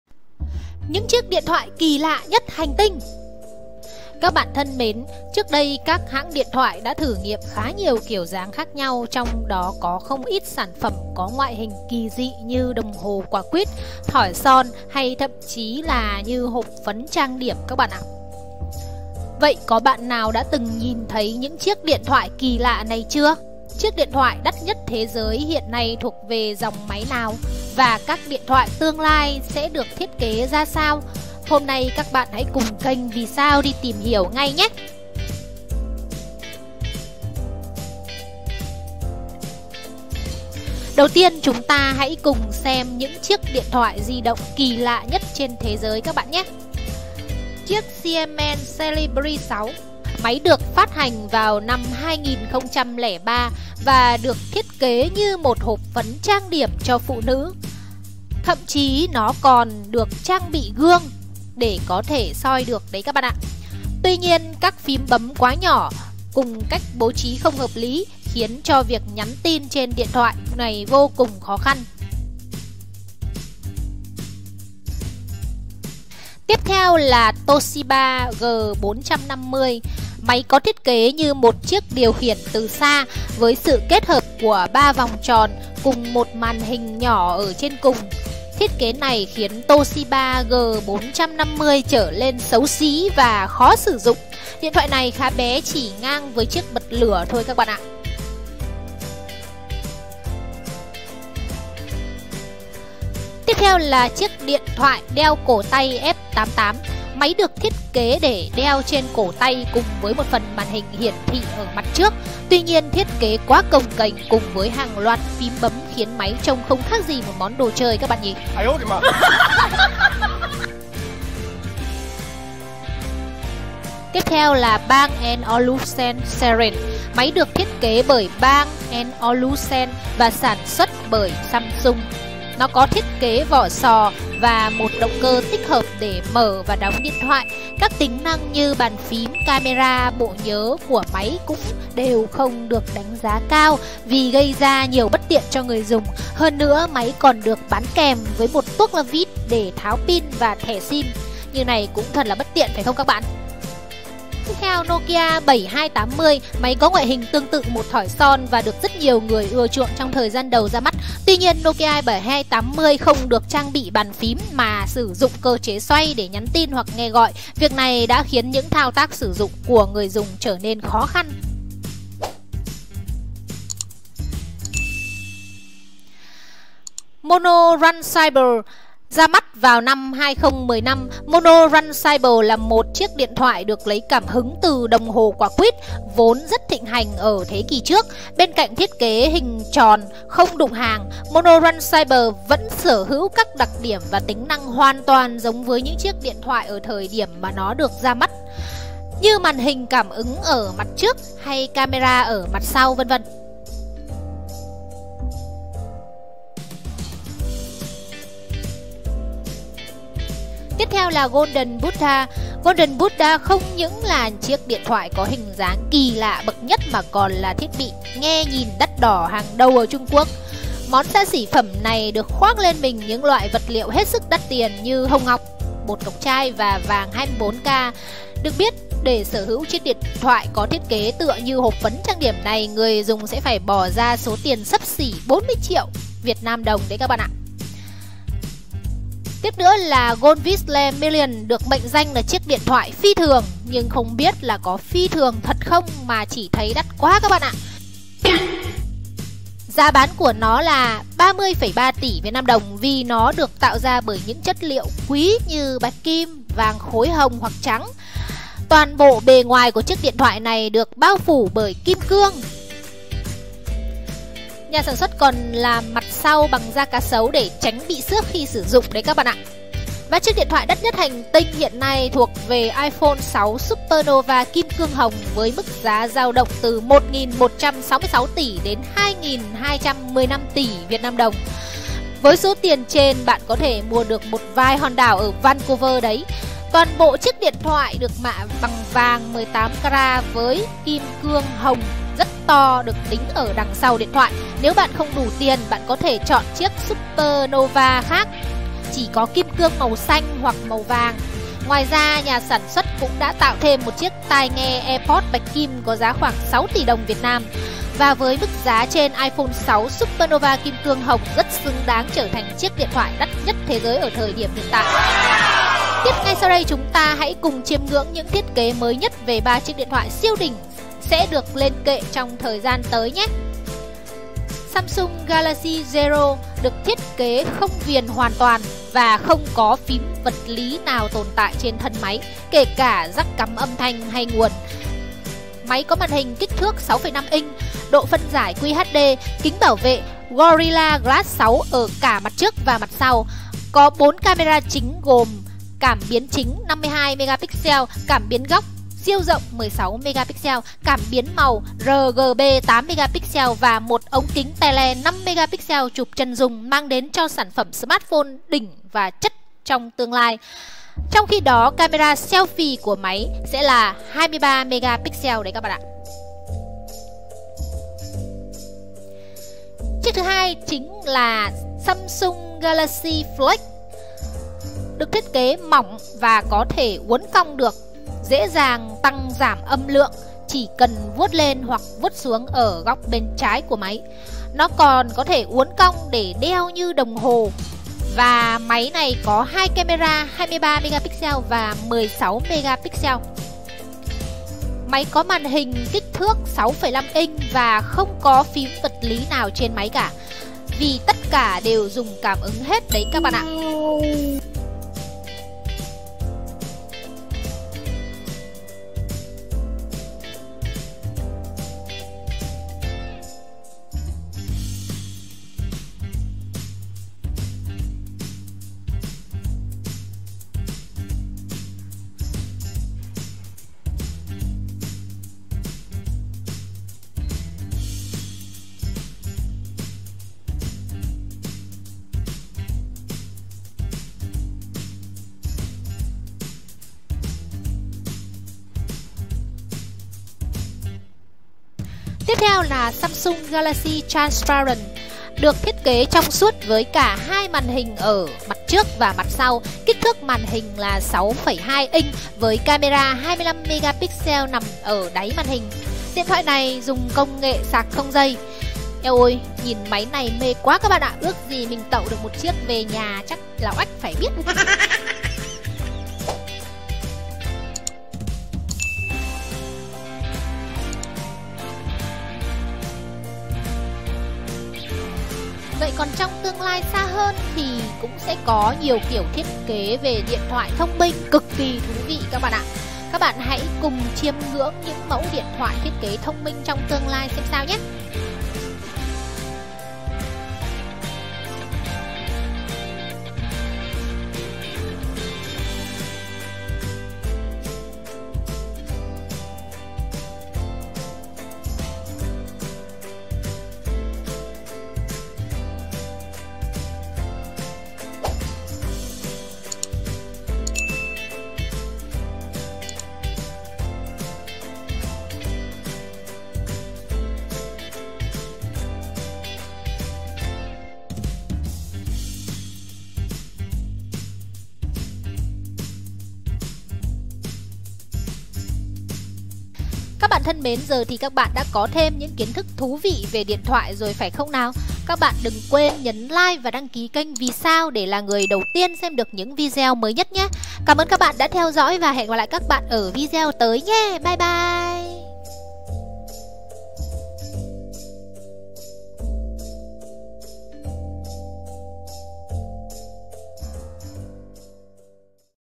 Những chiếc điện thoại độc lạ chắc chắn sẽ làm bạn ngạc nhiên | VÌ SAO
Điện thoại trong suốt như tấm kính, điện thoại có thể uốn cong trong tương lai trông sẽ ra sao? Các bạn hãy cùng kênh vì sao tìm hiểu nhé
Chúc bạn có thêm được nhiều kiến thức bổ ích cùng VÌ SAO nhé.